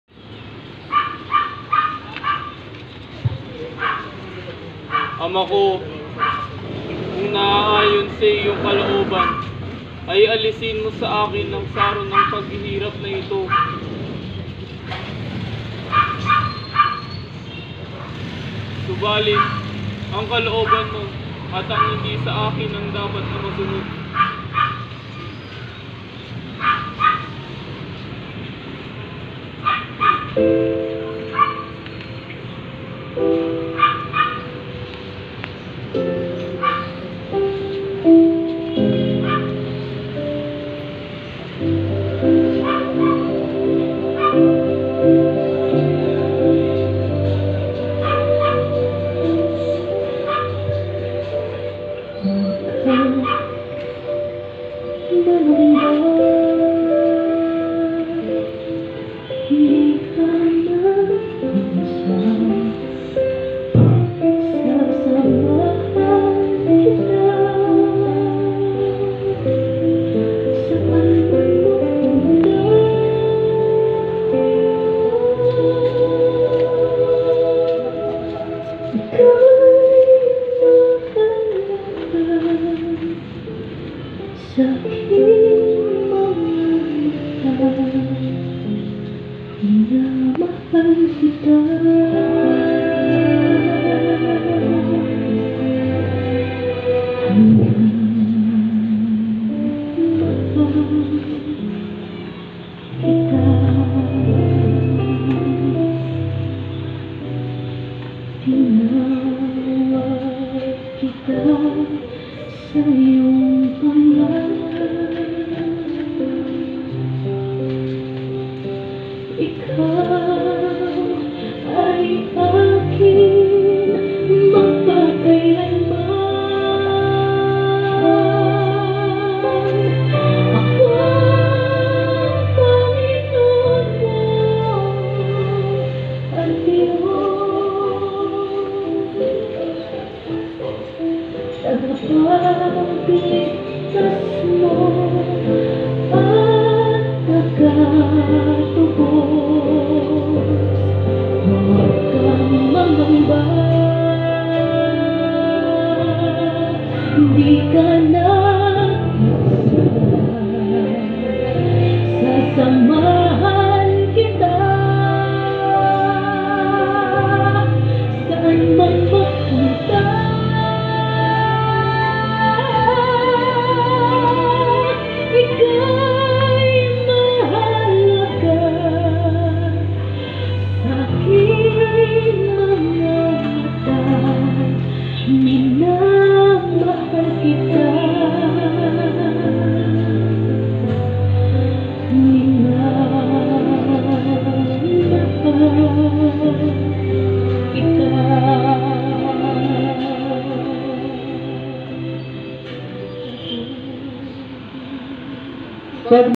Ikalawang Istasyon - Sa Hesus sa hardin ng Getsemani
24 Feb 2024
BEC Youth
Live Via Crusis - BEC Sector 6
(Ang Daan ng Krus 2024)

"Sinasamba ka namin at pinasasalamatan, Panginoong Hesukristo. Sapagkat sa pamamagitan ng iyong Mahal na Krus at Muling Pagkabuhay, iniligtas mo ang sandaigdigan."